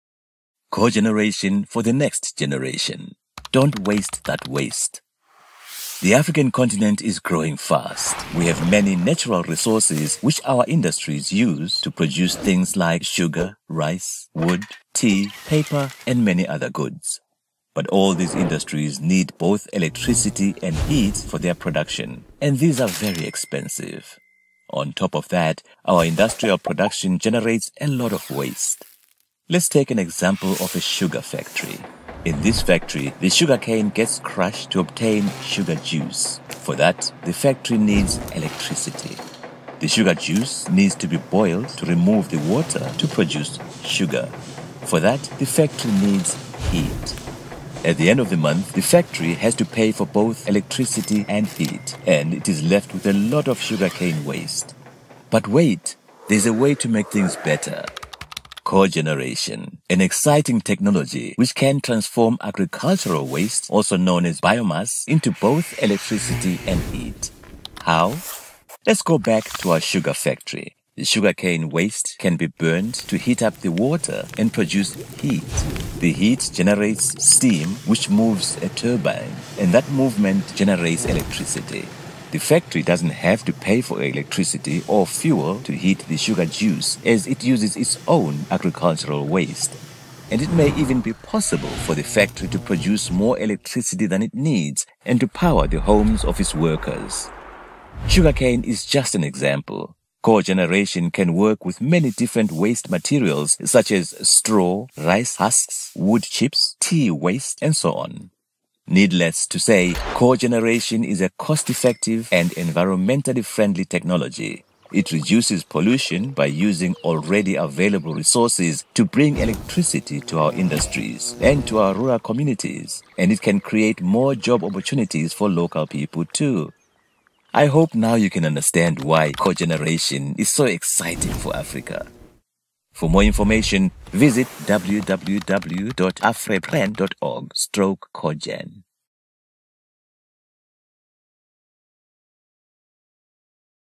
Cogeneration for the next Generation: Don't waste that Waste
The African continent is growing fast. We have many natural resources which our industries use to produce things like sugar, rice, wood, tea, paper and many other goods.

But all of these industries need both electricity and heat for their production.
And these are very expensive.